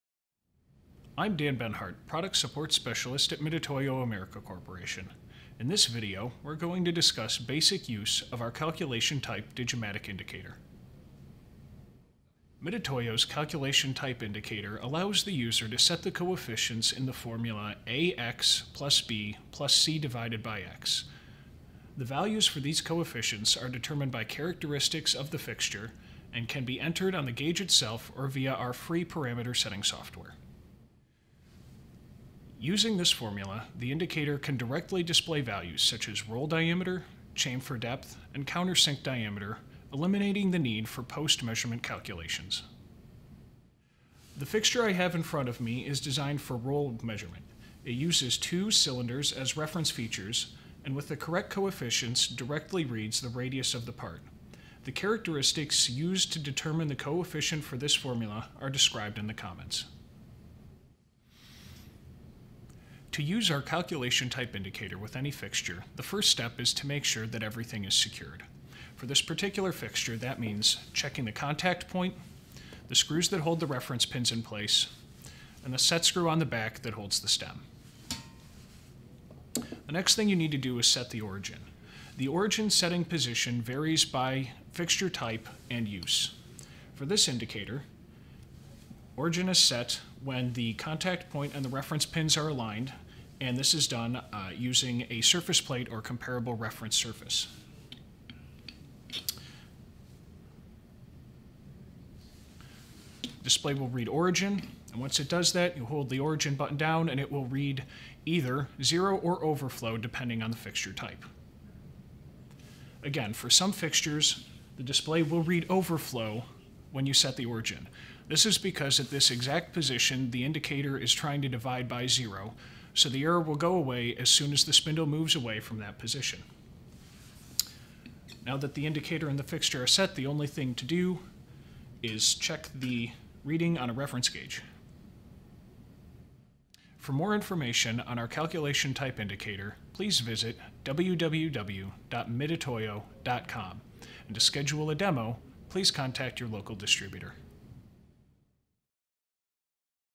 Mitutoyo 543-287B Absolute Digimatic Indicator ID-C Calculation Type
Mitutoyo 543-287B Absolute Digimatic Indicator ID-C Calculation Type; .5"/ 12.7mm Range , 3/8" Stem

The Mitutoyo 543-287B Absolute Digimatic Indicator ID-C is a conventional indicator that simply displays a spindle displacement but due to the Calculation Type Feature it incorporates an internal calculation function in place of spindle displacement. With features, measurements such as feeler, inside diameter and radius of curvature measurement can easily be obtained without the hassle of conversion tables or equivalents. It performs internal calculations using the formula Ax+B+C (assuming spindle displacement as x) while the specified coefficients A, B and C can be set with respect to the purpose of measurement or dimensions of the fixtures. This unique feature allow you to read your measurements directly, without fumbling for conversions. This model has a measuring range of .5"/ 12.7mm with a 3/8" Stem diameter and .00001"-.05"/0.0002-1mm switchable Resolution.

Features:
SPC data output
Switchable Resolution
Inch/Metric conversion
Calculation-Type to directly read measurements without conversion tables

For the Mitutoyo 543-287B Absolute Digimatic Indicator ID-C Calculation Type; .5"/ 12.7mm Range , 3/8" Stem Only, shop online now